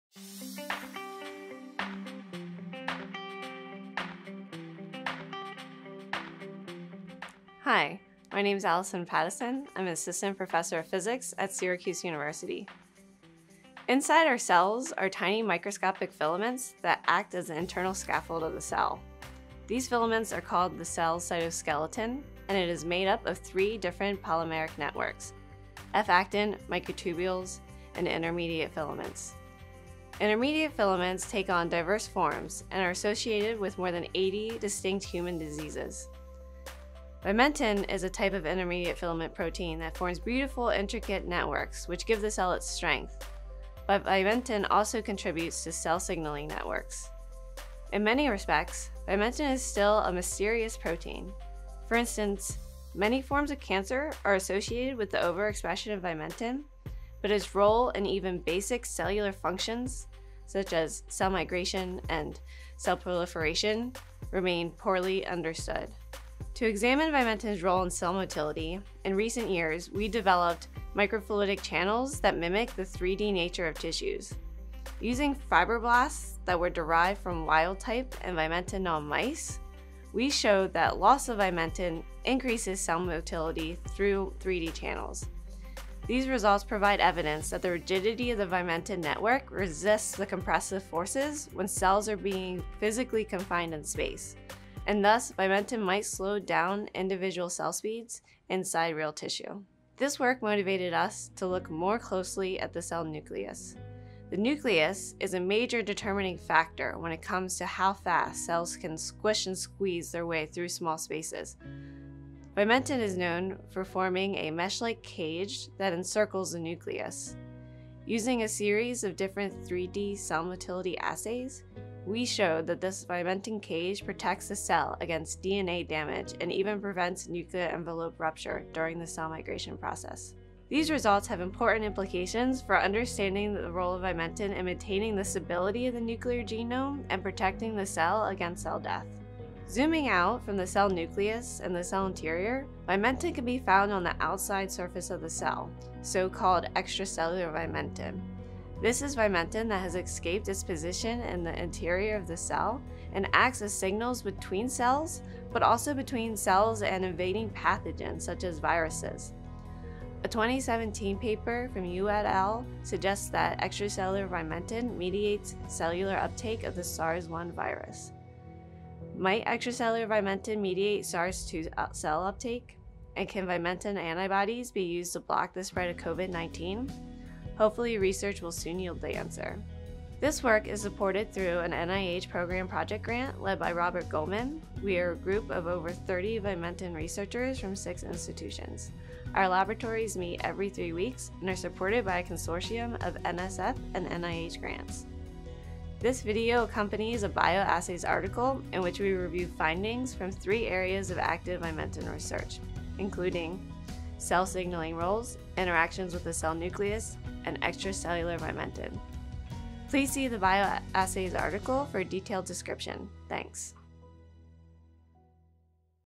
BioEssays: Mechanical and Non-Mechanical Functions of Filamentous and Non-Filamentous Vimentin
Vimentin forms a wickerwork network that provides mechanical protection of the nucleus and elasticity to the cytoskeleton. New work highlights vimentin’s life outside the cell. It is secreted by multiple cell types and can provide positive signals for wound healing and act as a cofactor for pathogen infection. For more, read the full article by Alison E. Patteson et al.